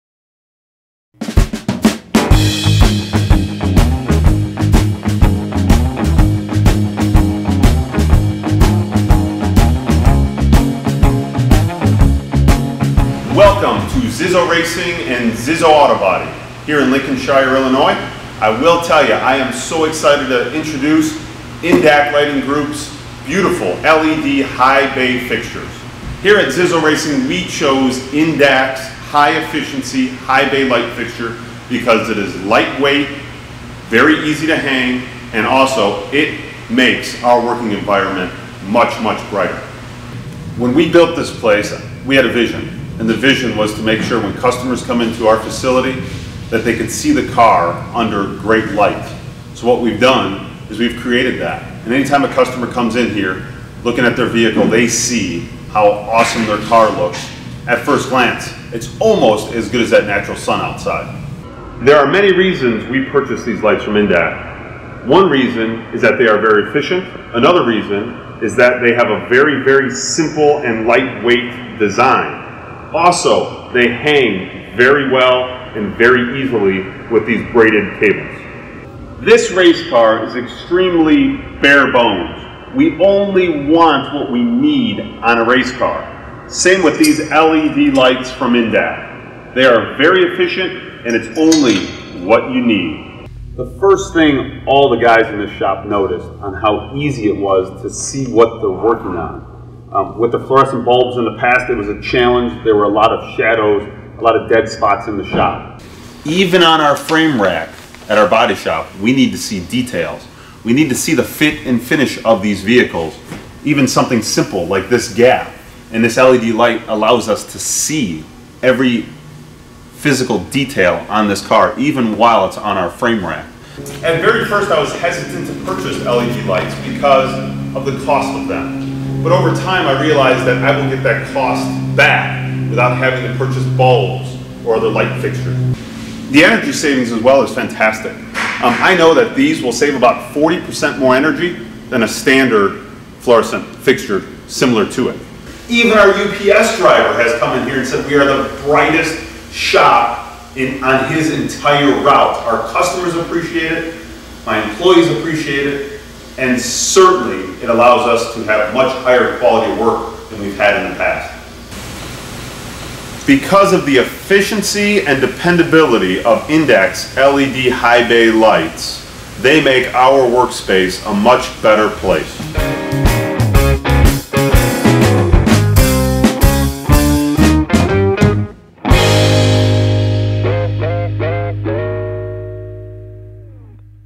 INDAK's LED "High Bay" High-Efficiency Lighting Fixtures
INDAK's LED "High Bay" High-Efficiency Lighting Fixtures at Zizzo Racing and Auto body

Bring the sunshine indoors! Check out INDAK's "High Bay" LED light fixture. Ideal for anybody requiring high intensity bright lighting!